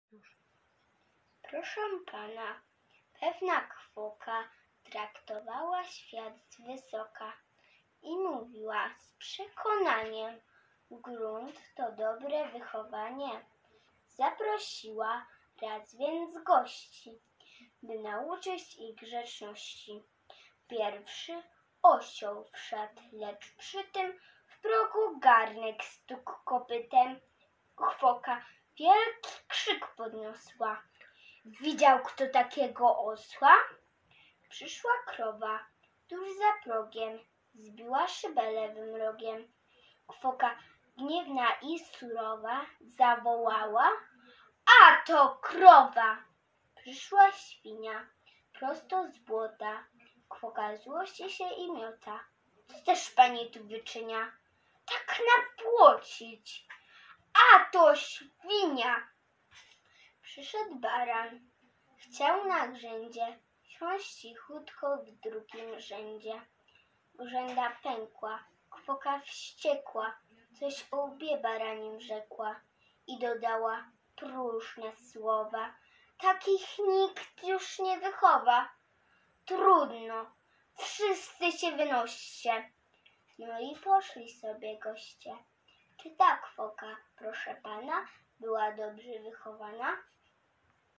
Kamila Fidos, PSP w Mroczkowie, "Kwoka" Jan Brzechwa
III Miejsce w Małym Konkursie Recytatorskim Online w kategorii klasy I - III